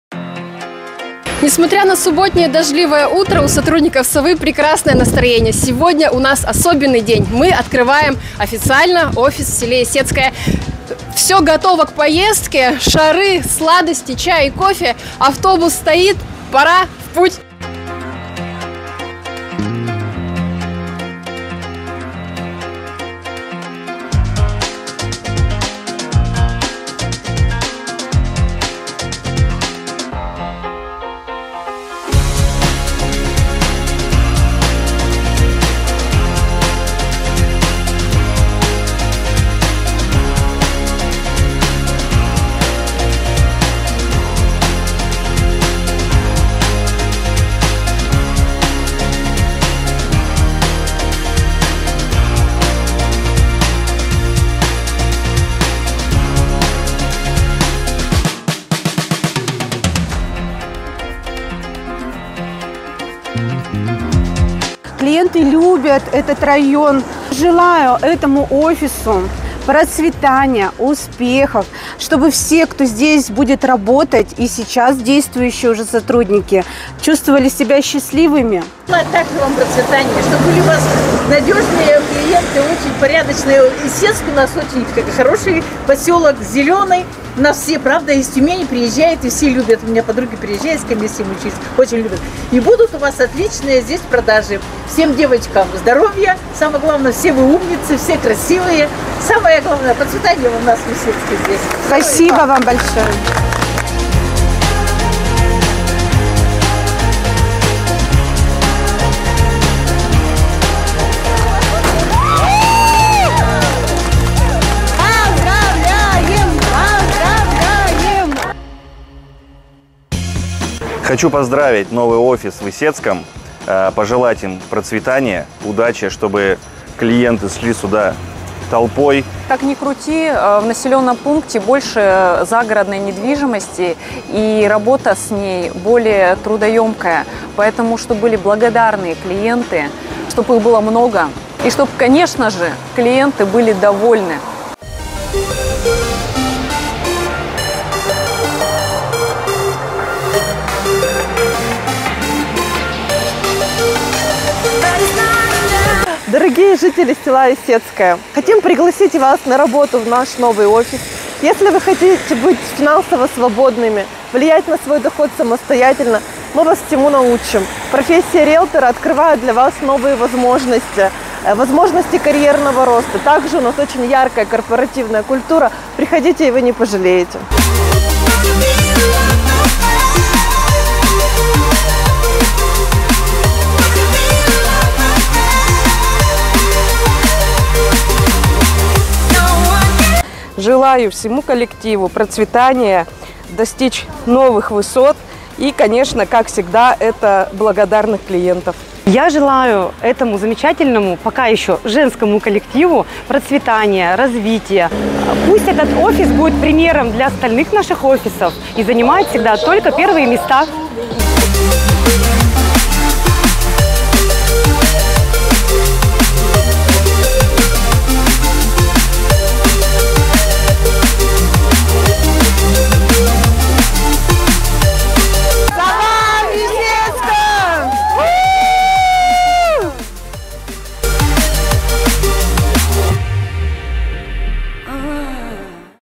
СОВА НЕДВИЖИМОСТЬ - Открытие офиса в селе Исетское
Ура! Этот день настал! Состоялось официальное открытие нашего офиса в селе Исетском! 🎉

На праздник приехали сотрудники, руководители допофисов, а также почетные гости из района и местные жители. Все вместе мы торжественно разрезали красную ленточку и оставили самые добрые пожелания новоиспеченным сотрудникам. А угощения и танцы с плюшевой совушкой подарили отличное настроение и зарядили радостью на предстоящие выходные! 💚

Приезжайте в новый офис «СОВА Недвижимость» по адресу: с. Исетское, ул. Гагарина, 1а. 📍

Совершайте покупку с нами, не беспокоясь о возможных рисках! 🦉